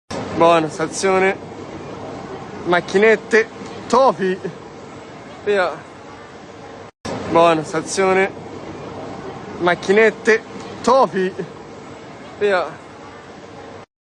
Firenze, spunta un topo nel distributore degli snack della stazione di S. M. Novella
Sorpresa e disgusto al distributore di snacks della stazione ferroviaria di Firenze Santa Maria Novella. Un video che sta circolando sui social e pubblicato dalla pagina Instagram «Welcome to Favelas» mostra un roditore che fa capolino tra due merendine.